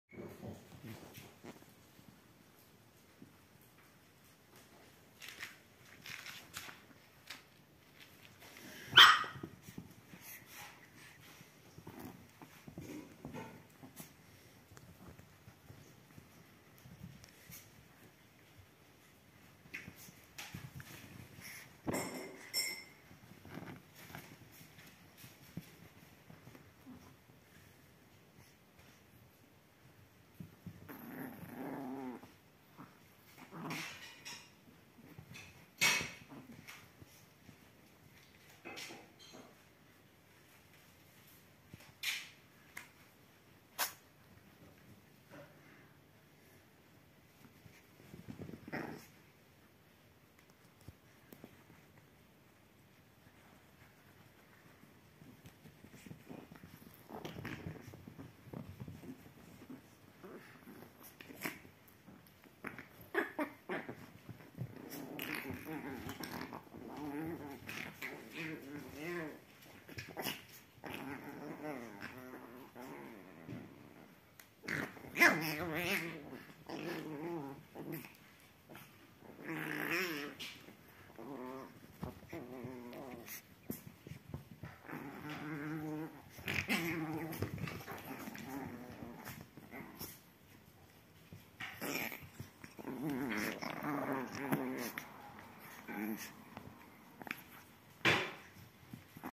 Very cute Chihuahua puppies play on the sofa.
Cute Chihuahua puppies have a great time playing and teasing eachother on the sofa.